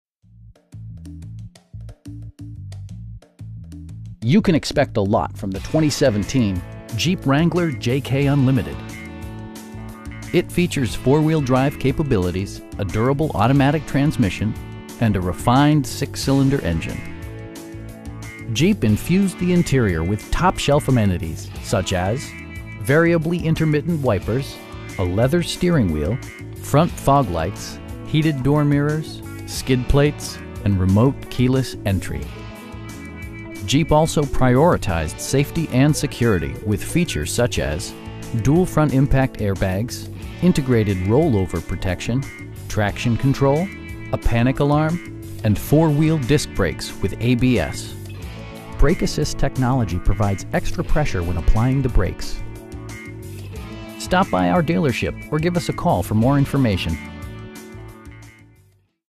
2017 Jeep Wrangler JK Unlimited Sahara 4x4
Davidson Nissan
18423 US Route 11

You can expect a lot from the 2017 Jeep Wrangler JK Unlimited. It features four-wheel drive capabilities, a durable automatic transmission, and a refined 6 cylinder engine. Jeep infused the interior with top shelf amenities, such as: variably intermittent wipers, a leather steering wheel, front bucket seats, front fog lights, heated door mirrors, skid plates, and remote keyless entry. Jeep also prioritized safety and security with features such as: dual front impact airbags, integrated roll-over protection, traction control, a panic alarm, and 4 wheel disc brakes with ABS. Brake assist technology provides extra pressure when applying the brakes. We pride ourselves in the quality that we offer on all of our vehicles. Stop by our dealership or give us a call for more information.